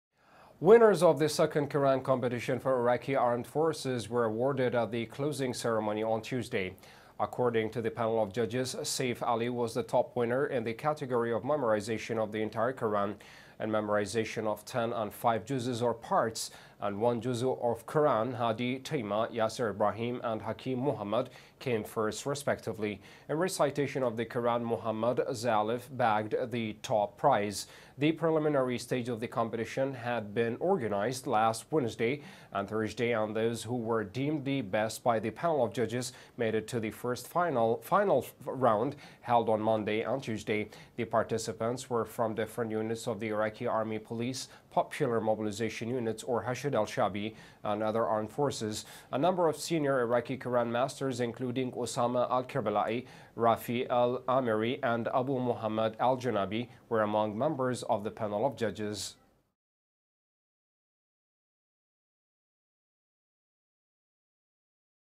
Iraqi Armed Forces Quran Contest Winners awarded
Winners of the second Quran competition for Iraqi armed forces were awarded at the closing ceremony on Tuesday.
According to the panel of judges, Saif Ali was the top winner in the category of memorization of the entire Quran.
In memorization of ten and five Juzes (parts) and one Juz of Quran, HadiTaima, YasirIbraheem and Hakim Muhammad came first, respectively.
In recitation of the Quran, Muhammad Zalif bagged the top prize.
The preliminary stage of the competition had been organized last Wednesday and Thursday and those who were deemed the best by the panel of judges made it to the final round, held on Monday and Tuesday.
The participants were from different units of the Iraqi army, police, Popular Mobilization Units (Hashd al-Shaabi) and other armed forces.
A number of senior Iraqi Quran masters, including Osama al-Karbalayi, Rafi al-Ameri and Abu Muhammad al-Jinabi were among members of the panel of judges.